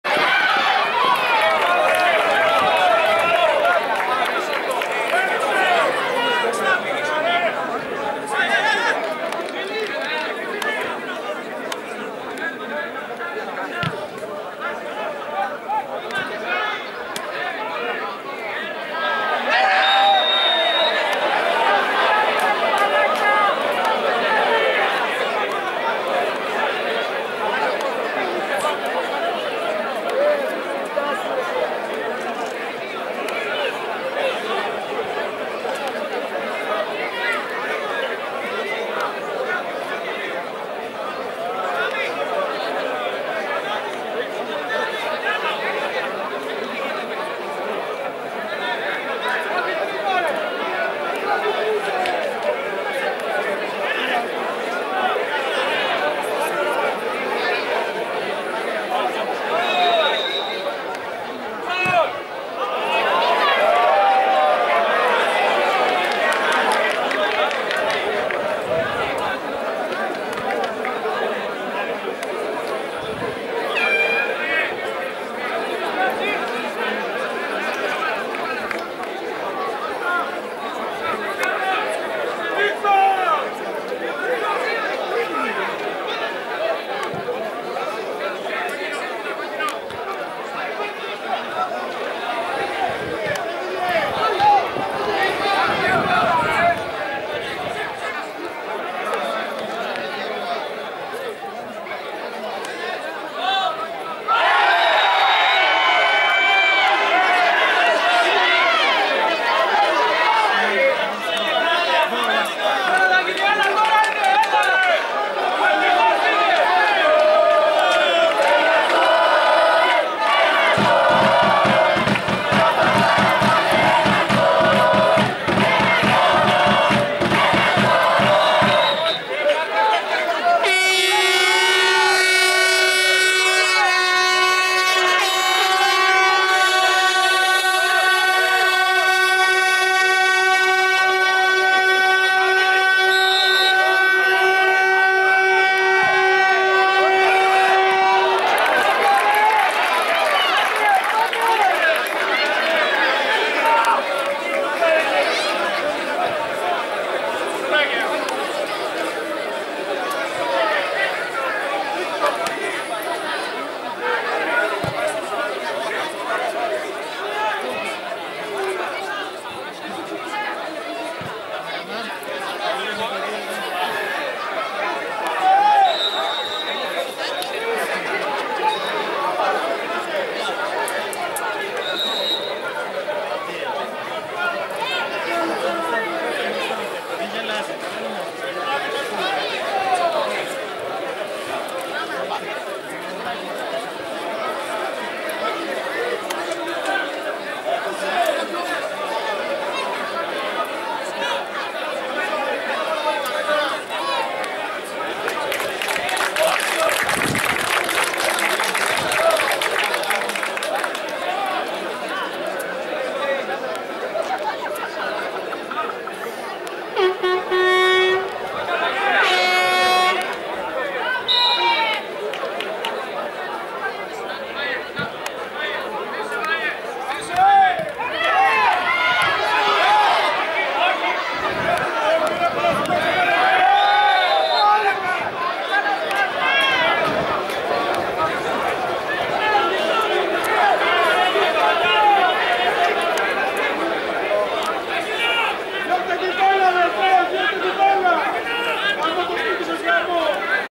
Ο.Φ.Α.Μ-Αστέρας Πετριτή-Πρωτ. Α1 Κατηγ. Ε.Π.Σ.Corfu,Φάσεις πριν τέλος Αγώνα,0-0,8-12--2019
Αυτό λίγο πριν το τέλος του αγώνα κι ενώ ο Ο.Φ.Α.Μ προσπαθεί να  σκοράρει-νικήσει και να αυξήσει την διαφορά των 2 βαθμών από τον Αστέρα Πετριτή,αλλά το σκόρ έμεινει ισόπαλο και ο..αγώνας συνεχίζεται για τον επιδιωκόμενο σκοοπό,κατάκτηση Πρωταθλήματος με βασικούς Διεκδικητές τις 2 αυτές ομάδες.!Corfu Football Championship for the 1st local Division between theTeams,F.C Aghios Mattheos(O.F.A.M)  vs F.C Astera Petriths.!Here moments of 2nd Half & before the end of the Game,it ended equaL 0-0.!